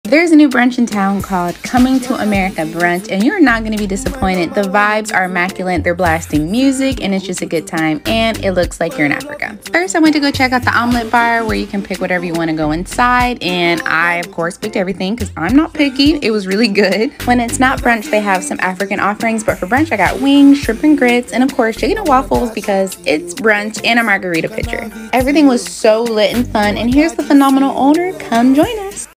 Coming to America Brunch (TikTok
Coming to America Brunch takes place at Bronze Kingdom Museum located at 6464 International Dr, Orlando, FL 32819